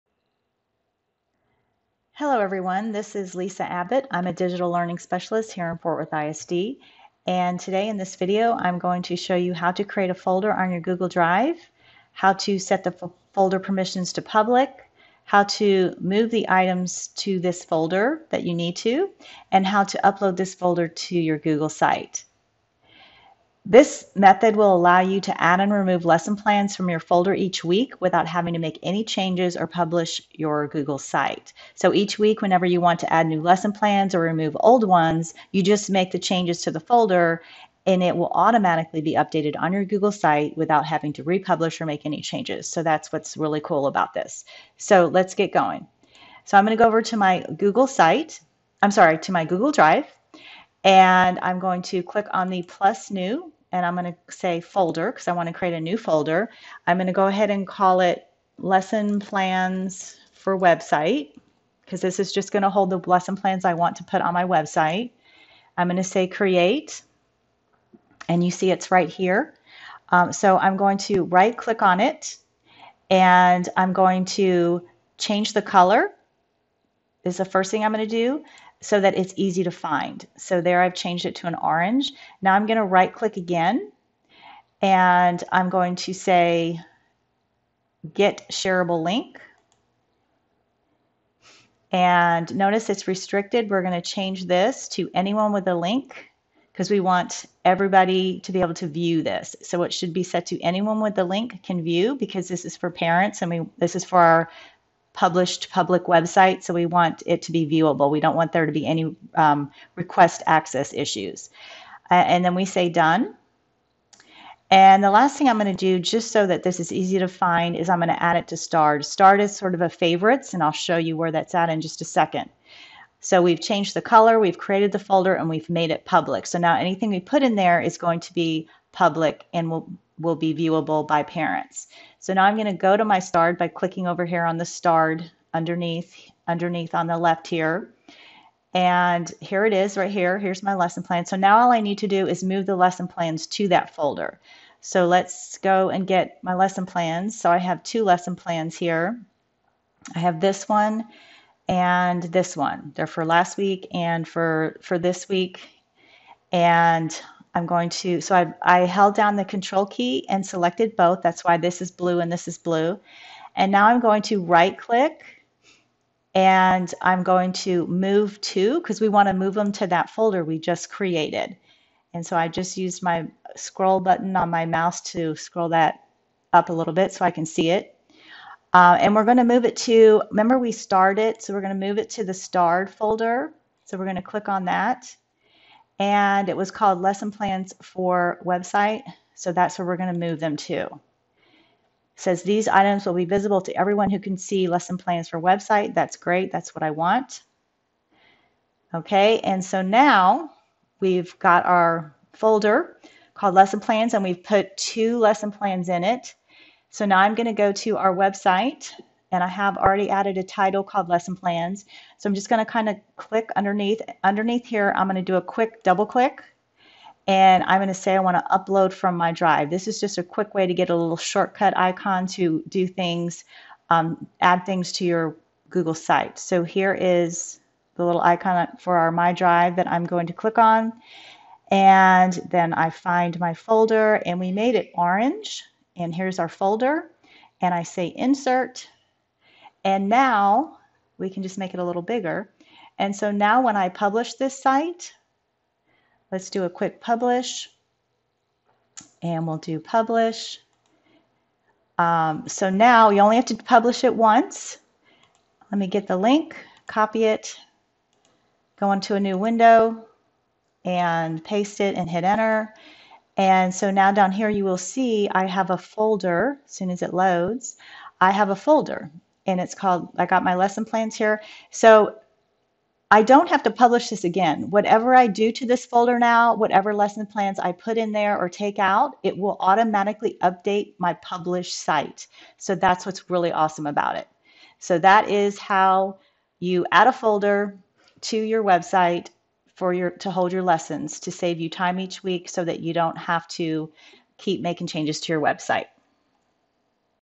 How to Add a folder to a Google Site
This video will show you how to create a folder on your Google drive, how to move items to your folder, how to make your folder public and how to upload the folder to your Google Site.